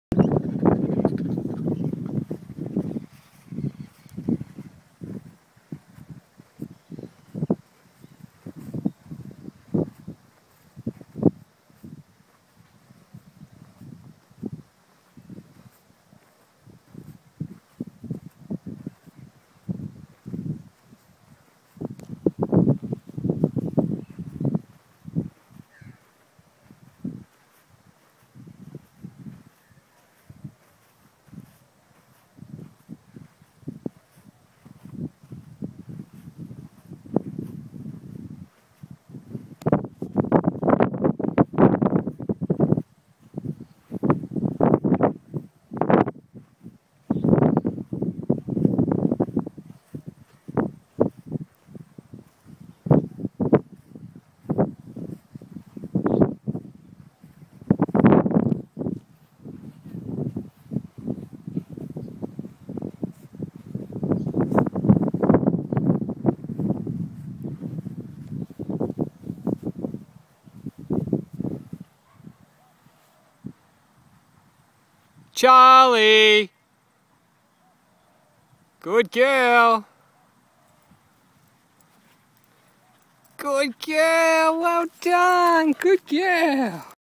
Charlie the kelpie -- recall
I was going through my photos on my phone and came across this. I had planned to post it awhile ago I appologise.
My wife and I took Charlie for a run at the local park and decided to do a drop step away to recall.